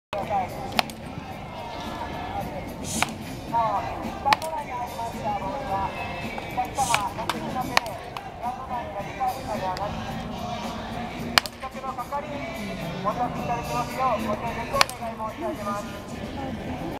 横浜DeNAベイスターズ バッティング練習をする佐野恵太選手
H31/4/4　神宮球場で試合前にトスバッティングをする佐野恵太選手。今日の試合では代打満塁ホームランを打ち、大大大ヒーローに。